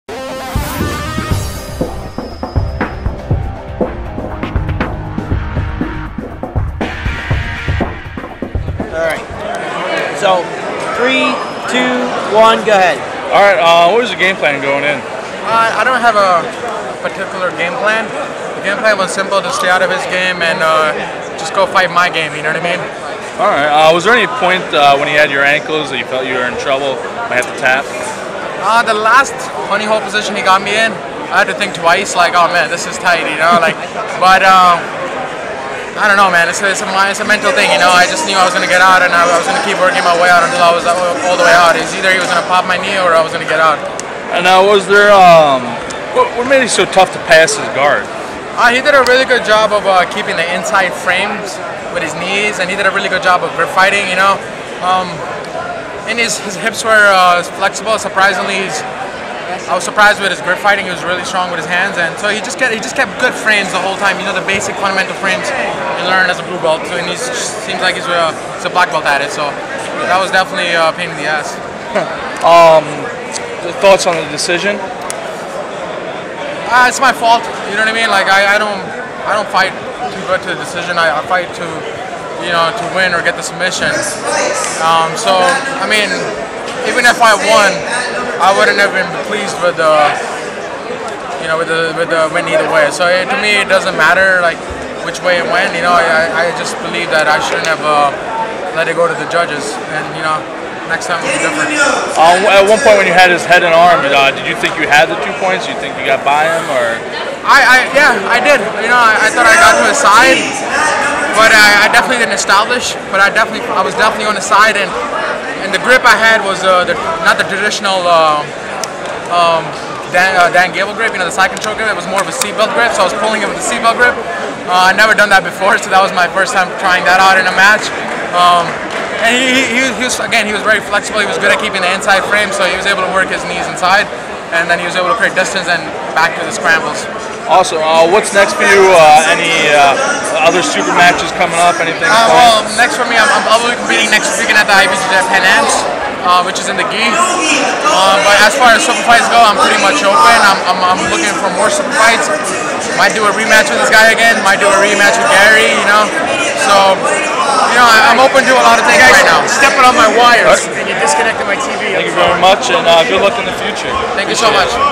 Mansher Singh Khera vs. Eddie Cummings - Grappling Industries post-match interview
Mansher Khera fell to Eddie Cummings via Referee decision after a 40-minute battle, where no points or advantages were scored.

Manhattan, NY
March 12, 2016